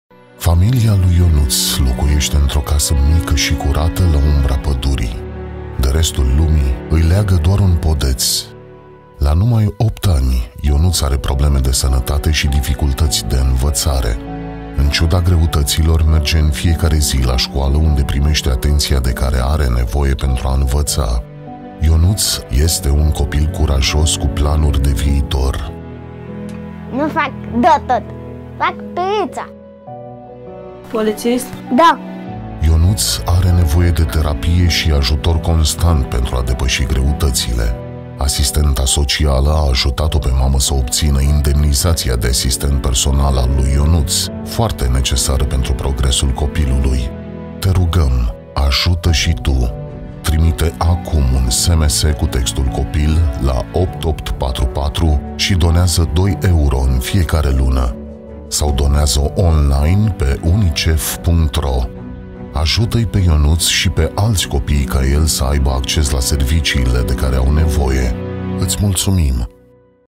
Ajută-l pe Ionuț și pe alți copii ca el!
Familia lui Ionuț locuiește într-o casă mică și curată, la umbra pădurii. Din casă, se aude în fiecare zi cum râd îngerașii. Ionuț este cel mai mare dintre ei și vrea să se facă polițist.

Dar nu e ușor.

Ionuț are o dizabilitate intelectuală și dificultăți de învățare, pe care le poate  depăși numai prin terapie și ajutor constant. Din septembrie, a început să meargă împreună cu fratele Cosmin la o școală nouă. O școală incluzivă, unde Ionuț primește atenția de care are nevoie pentru a învăța să citească și să scrie.

Progresele lui Ionuț și ale altor copii ca el depind de serviciile medicale, sociale și educaționale la care au acces.

Echipele comunitare locale pregătite de UNICEF oferă copiilor din familii vulnerabile accesul la serviciile de care au atâta nevoie.

Te rugăm, ajută și tu! Donează acum: Ajută-i pe Ionuț și alți copii ca el

Îți mulțumim!